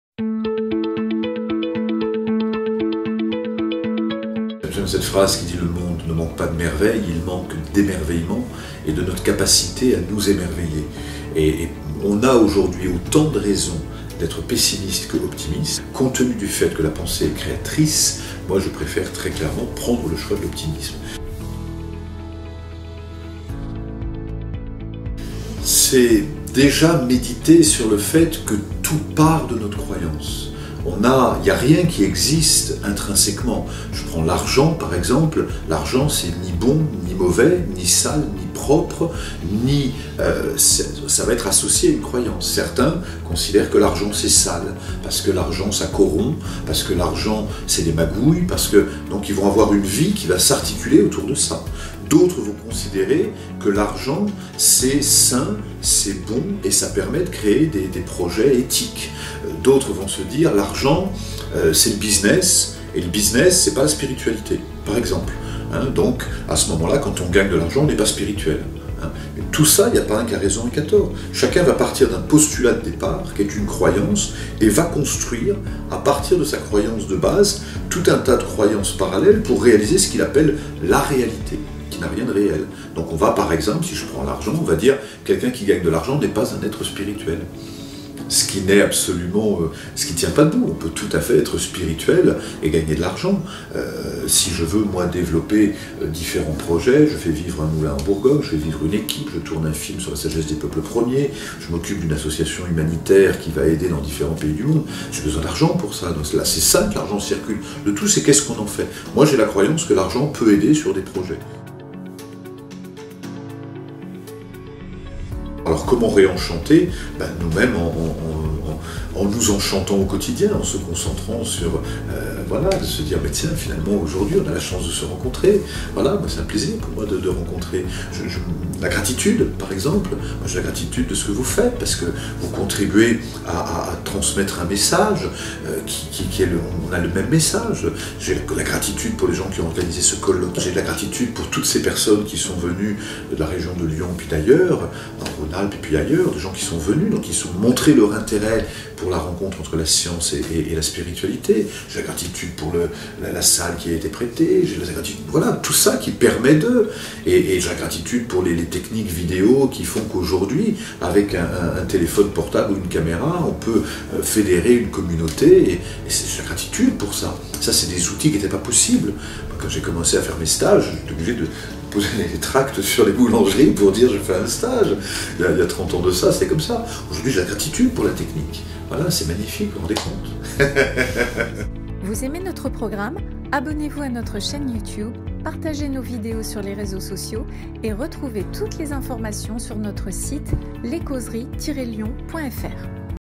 Arnaud RIOU " Ré-enchanter votre monde"
Comment travailler sur nos a priori qui conditionnent nos comportements et renforcent nos croyances ? Comment au quotidien adapter notre posture et enchanter notre monde intérieur en dépit des événements qui nous échappent ? Via la gratitude nous dit Arnaud Riou...